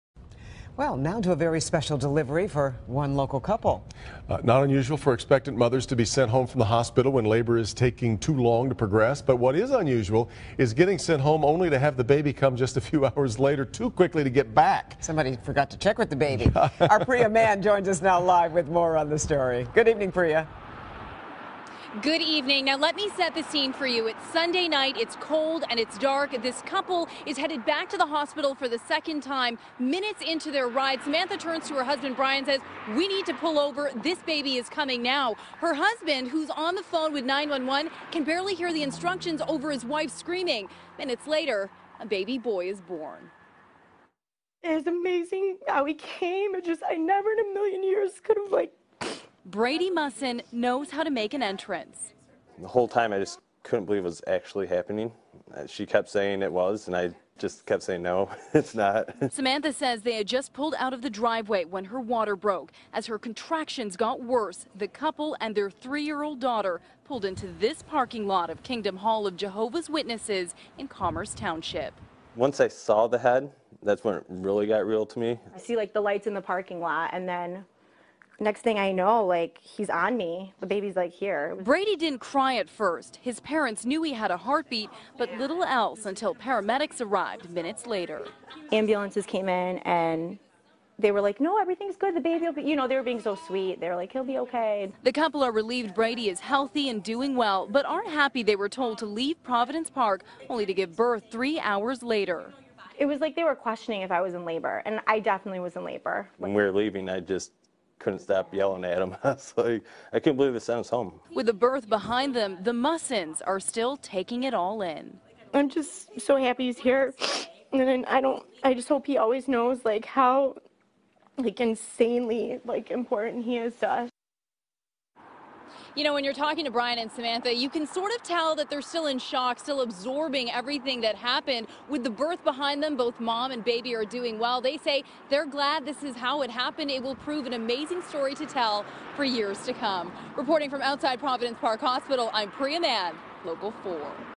911 Call: Mom Safely Delivers Baby in Commerce Township Parking Lot
Highland Township family welcomes new baby boy in SUV

A 911 call doesn't always have a happy ending, but this one does.

Samantha Mussen went into labor on Feb. 16 and rushed to Providence Park Hospital, but the family was told they could leave. But once back at home, the Mussen's son was ready to make his debut.

Samantha, her husband, Brian, and their 3-year-old daughter got back into the car and started the drive back to the hospital -- but there wasn't enough time.

"The whole time I just couldn't believe it was actually happening. She kept saying it was and I kept saying, 'No, it's not," Brian told Local 4.

The family pulled into a Jehovah's Witnesses Kingdom Hall parking lot and called 911, but the baby's head was already showing.

"The baby was just born ... I just delivered him here in the parking lot," Samantha tells the 911 operator.

The operator asked Samantha is she had any complications and if the baby was breathing. Samantha said the baby didn't cry, but was breathing and she was fine.

"He's here," Samantha is heard telling her daughter on the 911 call.

Paramedics arrived and made sure the baby, whose name is Brady, was healthy.

"They said everything's good. They were being so sweet. They were like, 'He'll be OK,'" Samantha said.